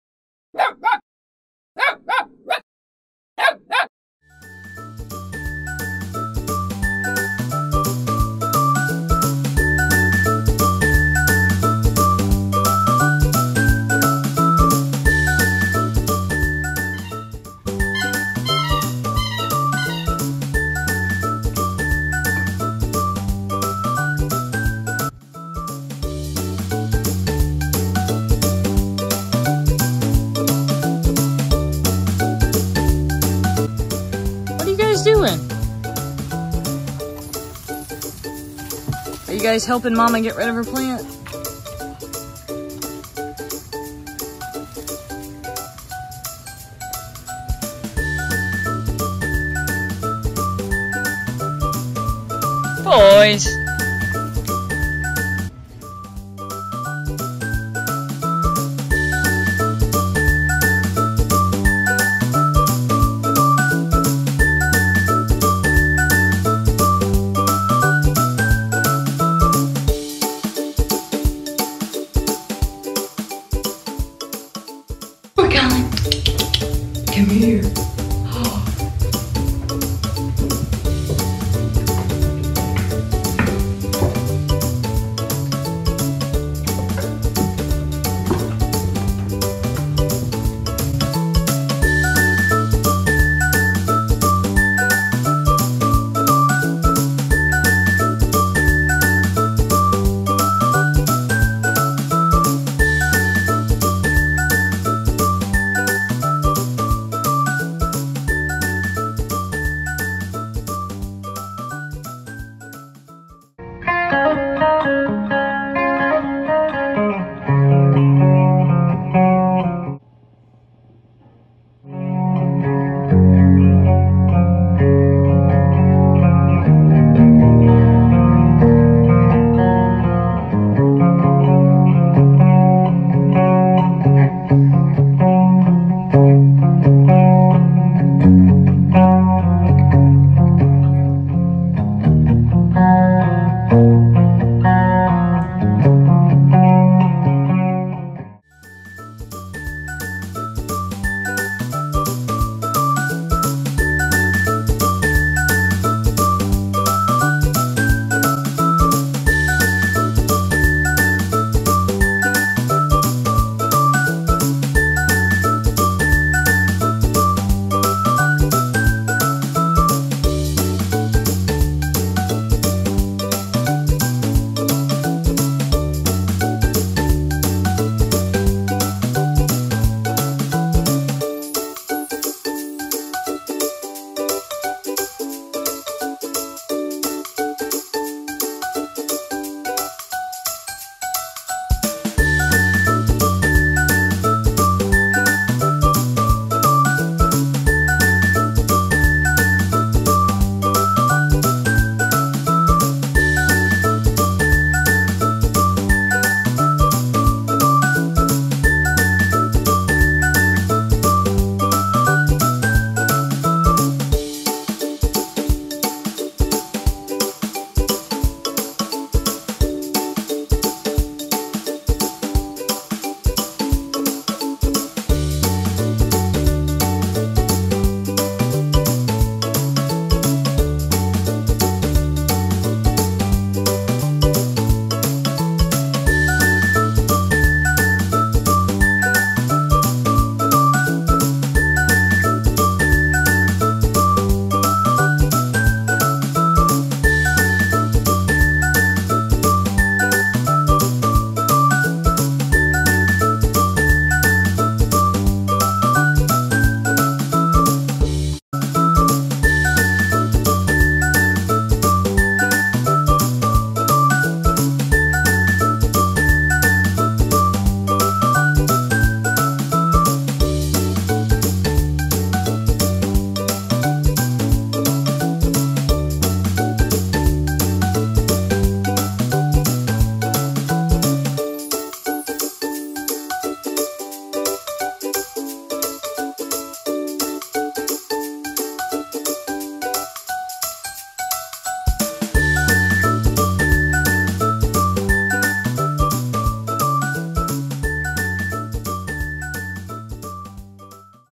Cooper and Collin's fun, fun, fun, fun world.
Even the ordinary can be extraordinary when you're a cute Berner pup! From nap time to yard work, these two Bernese Mountain Dog puppies definitely know how to make any task fun!